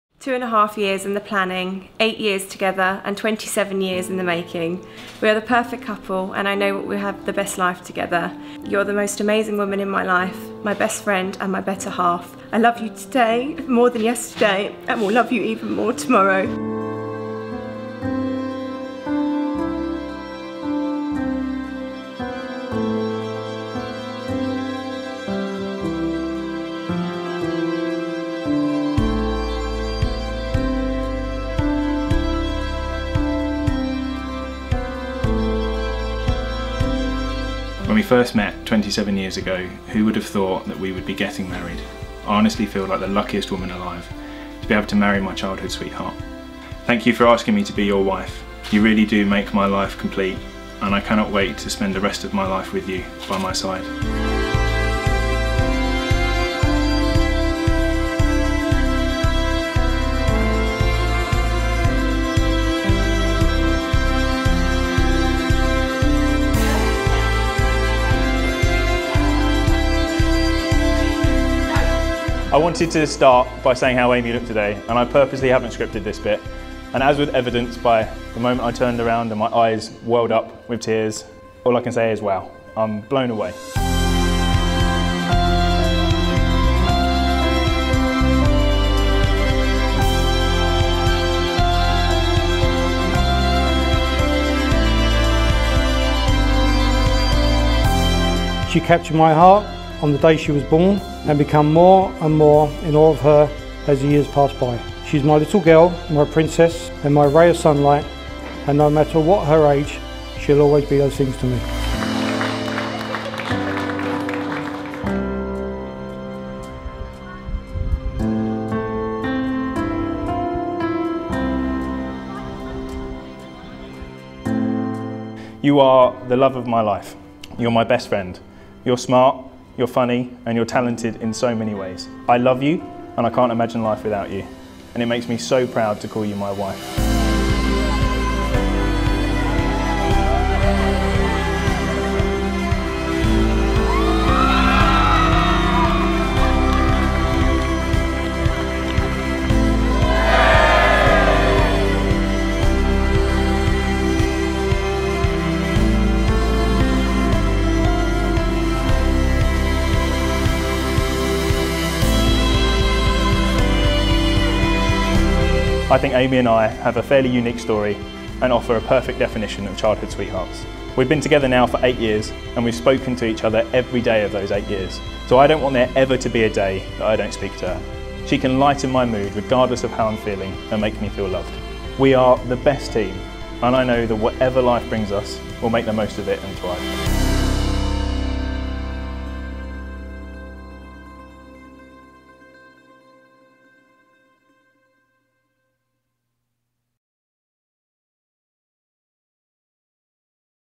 Cooling Castle wedding video / Amie + Jonathan
A Cooling Castle wedding video. Cinematic wedding video produced by NDR films.